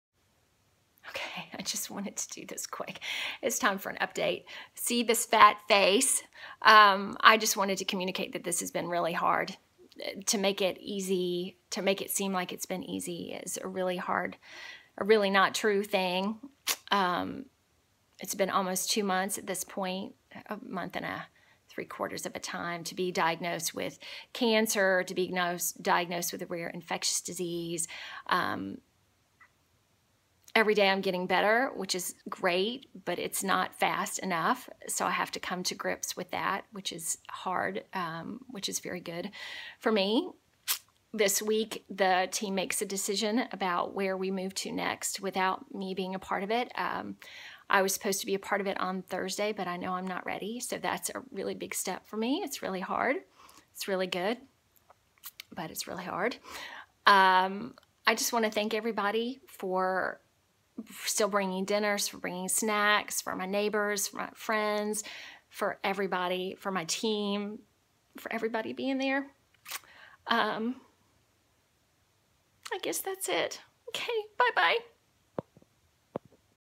July 24, 2016 - 2 Weeks-ish Being Home - Ooo This Is A Hard One
My name is Eden Koz. I was diagnosed (at least with 95% assuredness) with Stage 4 Lung Cancer which turned out to be (after a month) a rare, rare infection called nocardia beijingensis. The infection entered my lung through the breath and ended up creating 3 abscesses in my brain as well as the original one in my lung. I ended up having two craniotomies and almost died twice. This is one of the series of videos that I did for family, friends and business loves... capturing the challenges and beauty of this.

My last video was July 7. Wooo this is serious and honestly, probably the only time through all of this that I was really upset. Through all of this. Language is good. I am still getting an IV 3 times a day. And, yeah, that is a whistle around my next. And, everything with my business was also changing and altering because of this infection. At this point too, I just didn't feel good a large majority of the time. Was it ever going to end, I had really started asking myself.